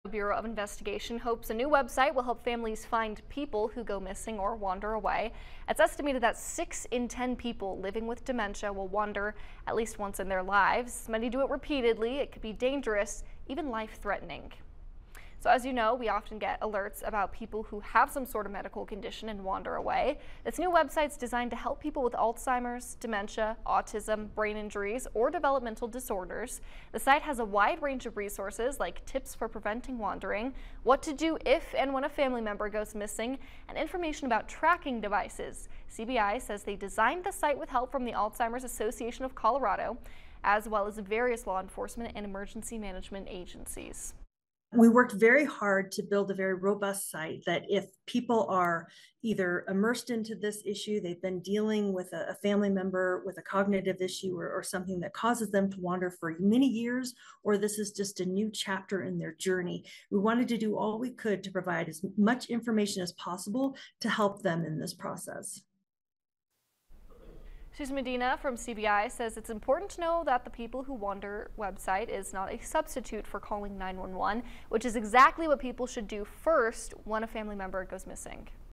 ‘People Who Wander’ website launches
The website is meant to help find lost people with medical conditions